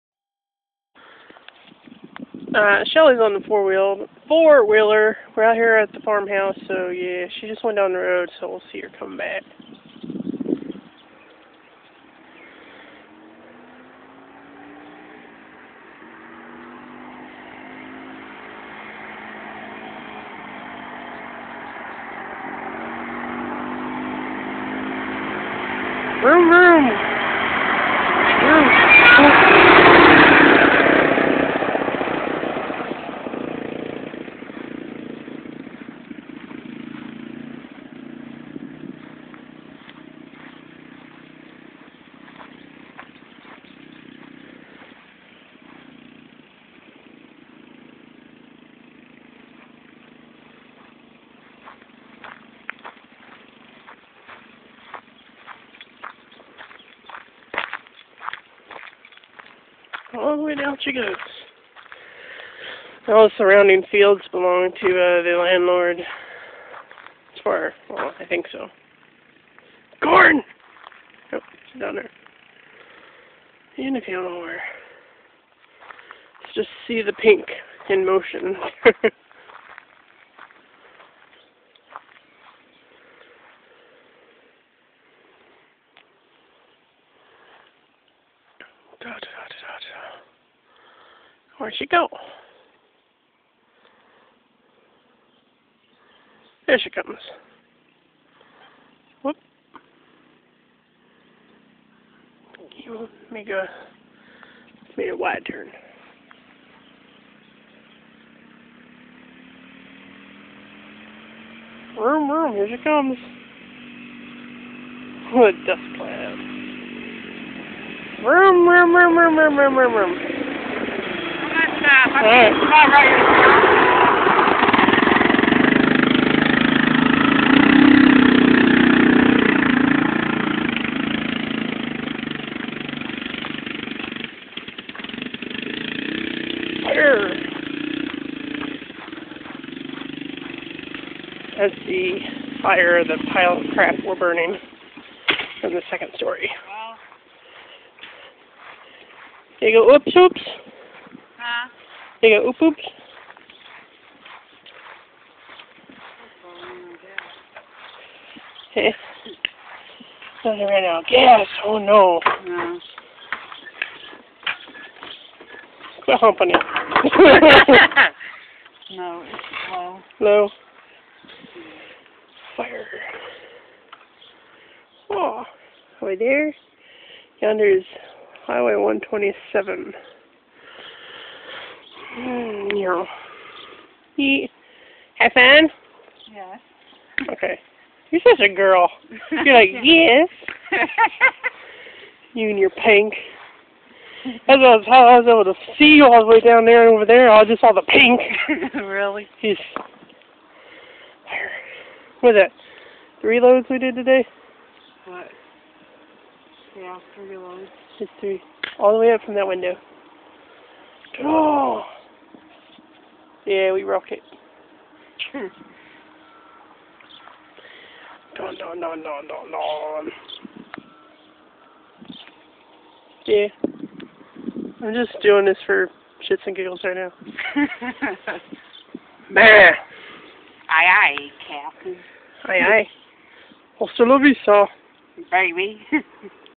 ATV Zoom zoom! [August 8th, 2013]
Shell-Kat decides to have a little fun on the 4 Wheeler.